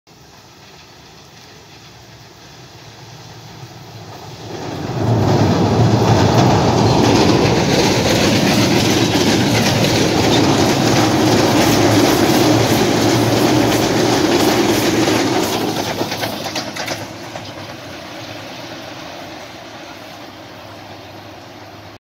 Supervia-Trem CNR série 3000 em Japeri com origem da Central, e com destino à estação de Japeri RJ.
Trem: Chinês
Série: 3000
Local: Japeri RJ
Origem: Central do Brasil
Destino: Estação de Japeri RJ.